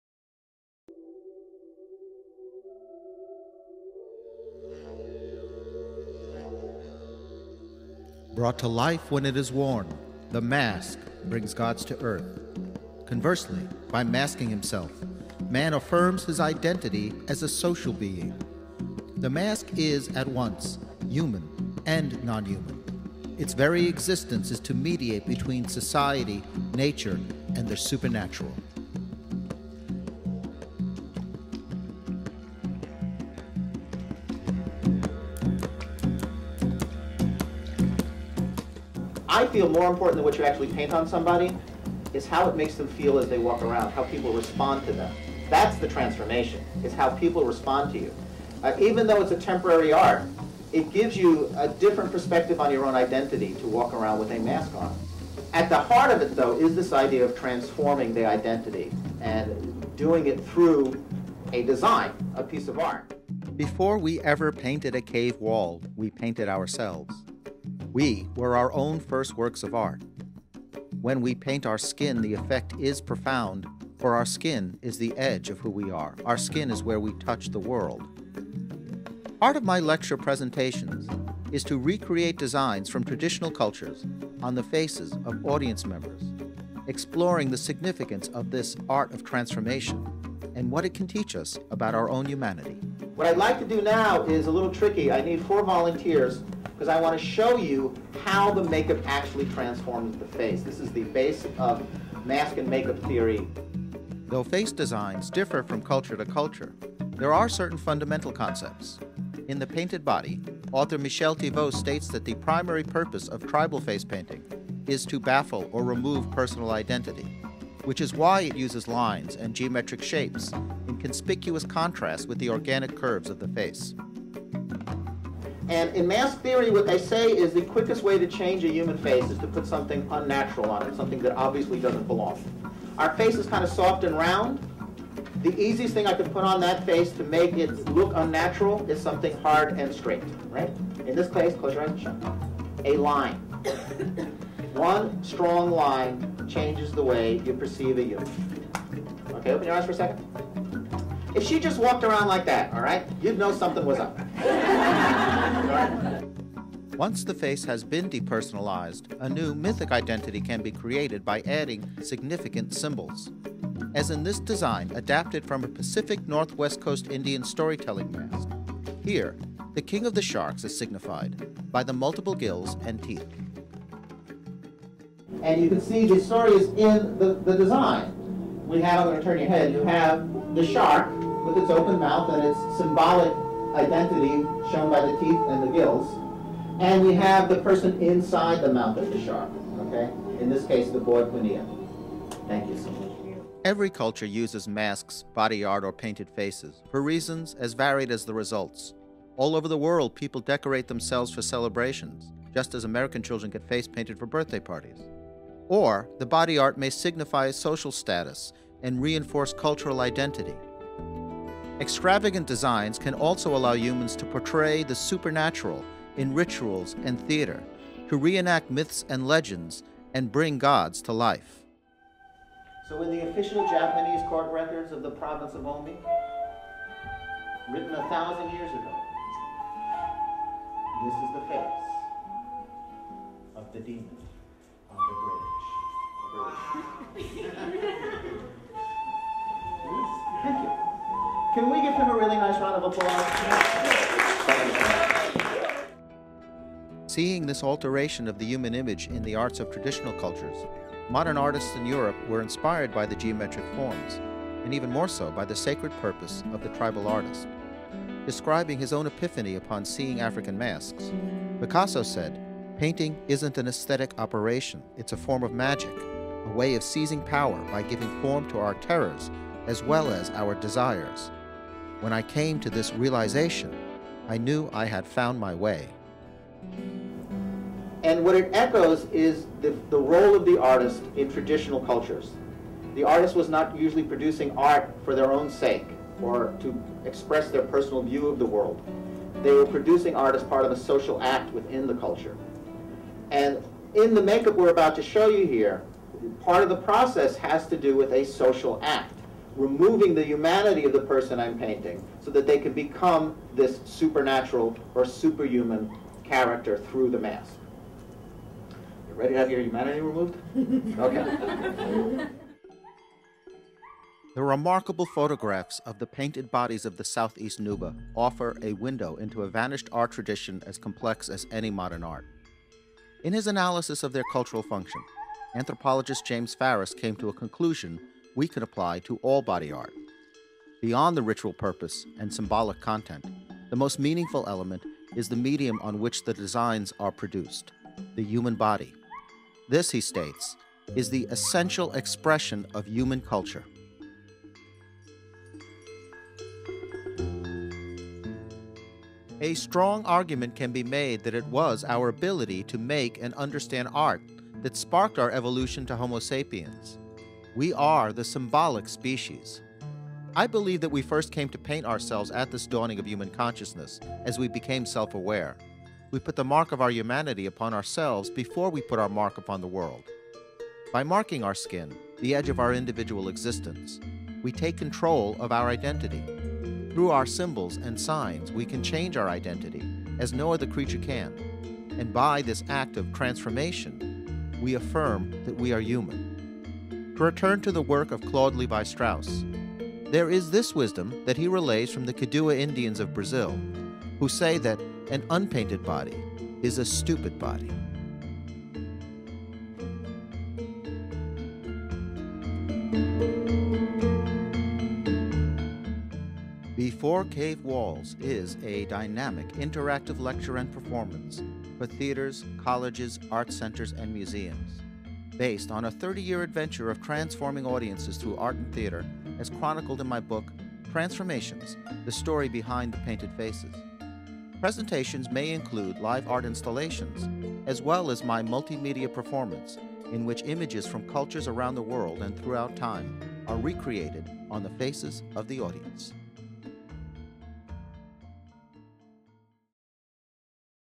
Before Cave Walls...A Dynamic Lecture from AgostinoArts
A distillation of Christopher Agostino's multimedia stage presentation on the art of transformation - the universal human art of using masks, makeup and body art to transform our identity - and what this art can teach us about who we are.